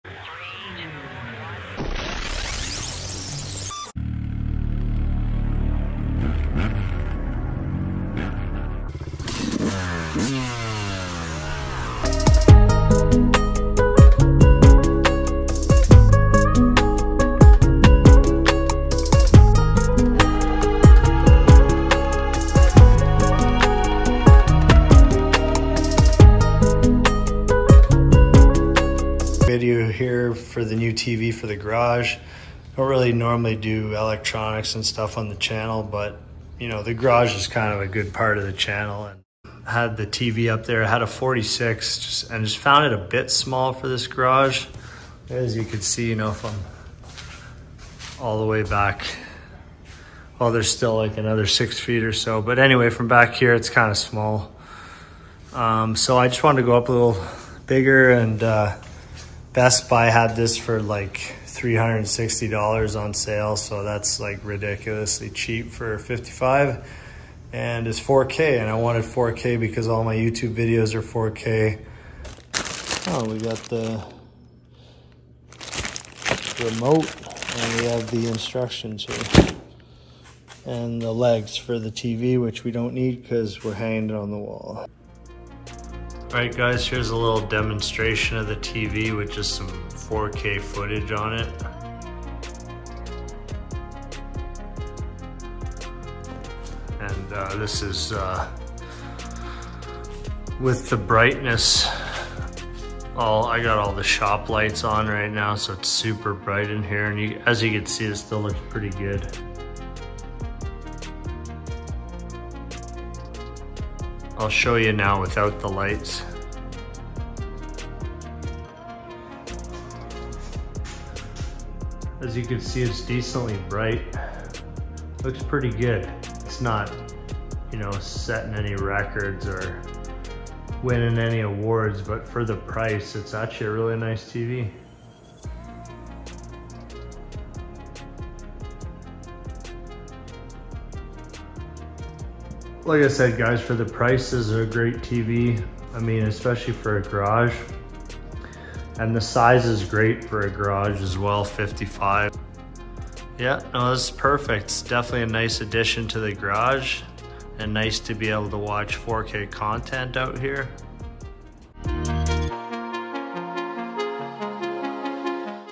TCL 55 Inch 4K TV Perfect for the GARAGE
The TCL 55S450G 55 4k Google Smart Tv is a great value for the price and in my case it made a great tv for the garage!